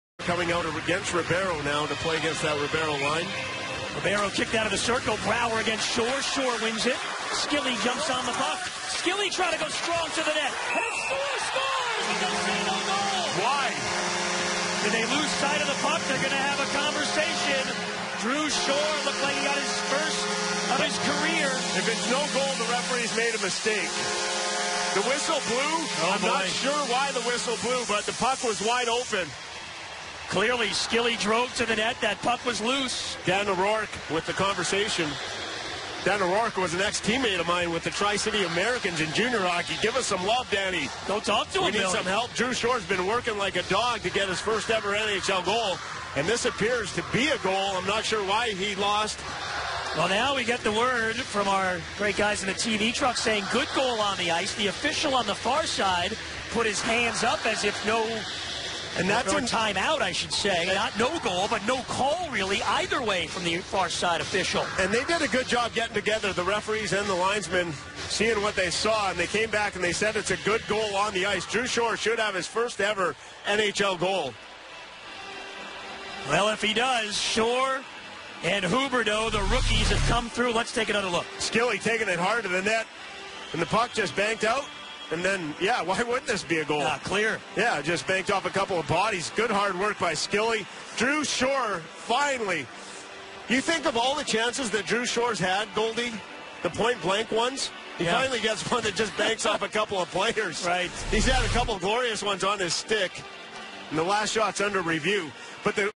Drew Shore scores first NHL goal after crazy deflections
Drew Shore's first NHL goal, with an assist to the hockey gods.